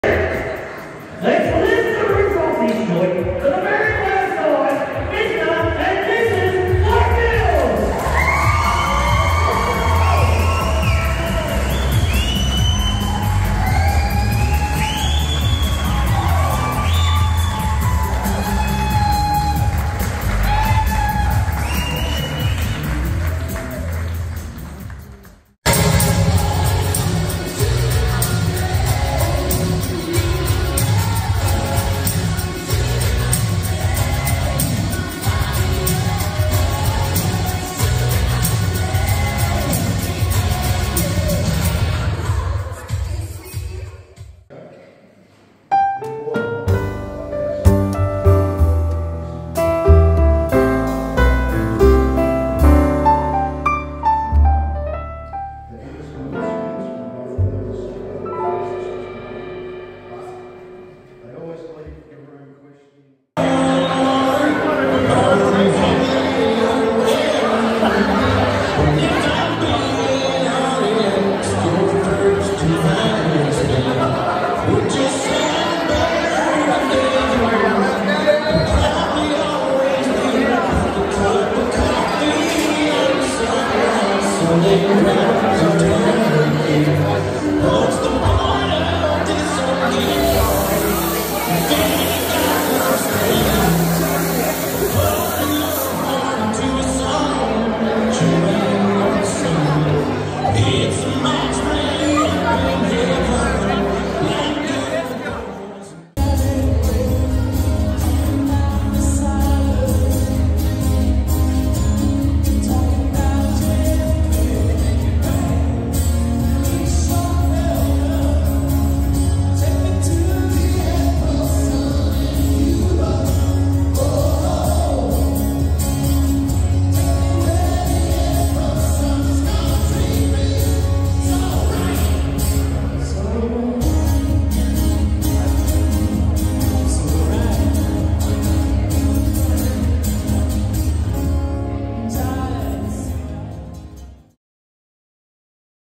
Out Of Rock DJ Acoustic Rock Duo Hobart Tasmania (Wedding Party)
Wedding party at The Hidden Gardens Hobart